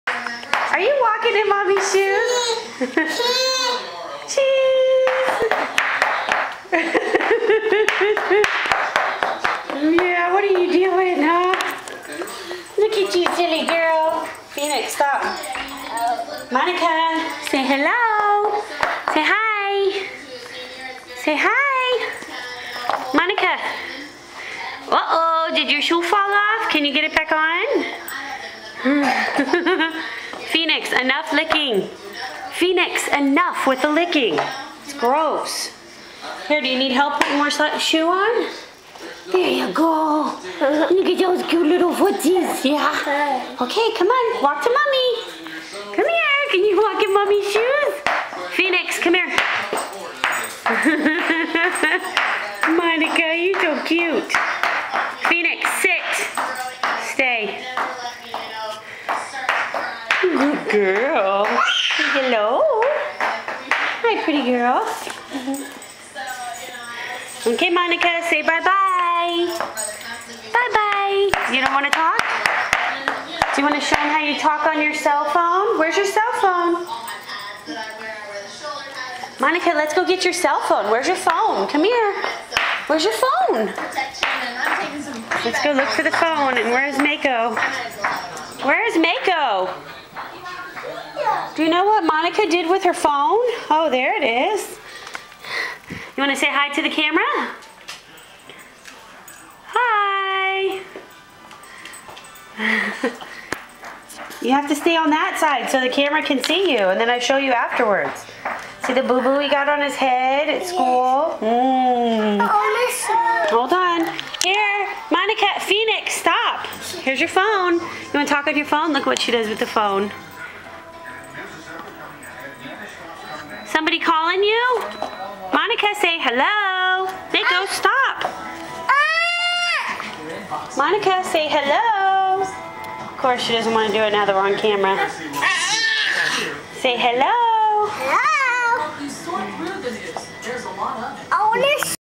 Monica in mommy's shoes.avi
walking in Mommy's shoes, and then Mako wants to get on camera too